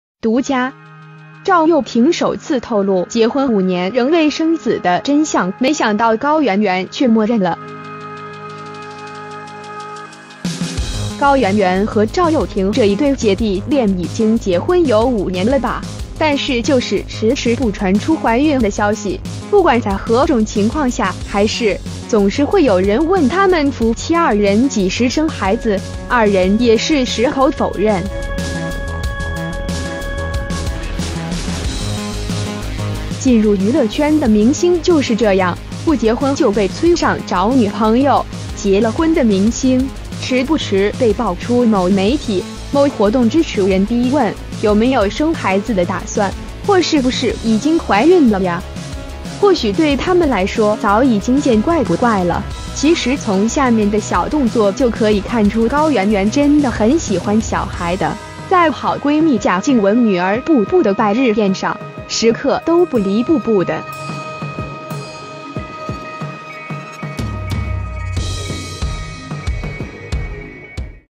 獨家！！趙又廷首次透露「結婚5年仍未生子的真相」沒想到高圓圓卻默認了！
趙又廷終於道出結婚5年仍未生子的真相，沒想到高圓圓卻默認了！